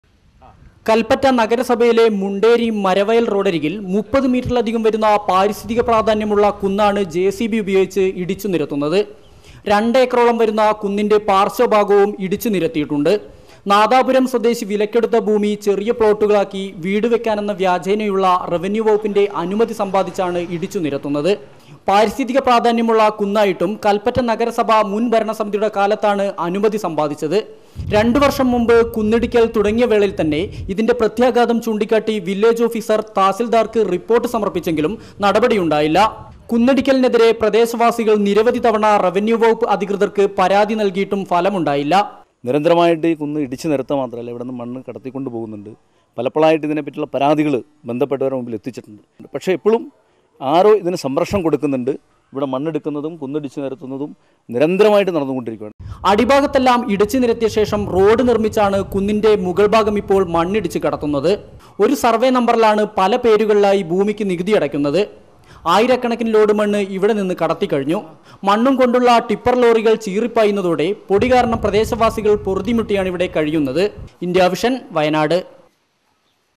Hill Demolishing for building Flat Complex
വയനാട്ടിലെ കല്‍പ്പറ്റ മുണ്ടേരിയില്‍ നിയമം ലംഘിച്ച് കുന്നിടിച്ച് മണ്ണുകടത്തുന്നത് സജീവമാകുന്നു. ഫ്‌ളാറ്റ് സമുച്ചയം നിര്‍മ്മിക്കുന്നതിനാണ് പാരിസ്ഥിതിക പ്രാധാന്യമുള്ള കുന്ന് ജെസിബി ഉപയോഗിച്ച് ഇടിച്ച് നിരത്തുന്നത്.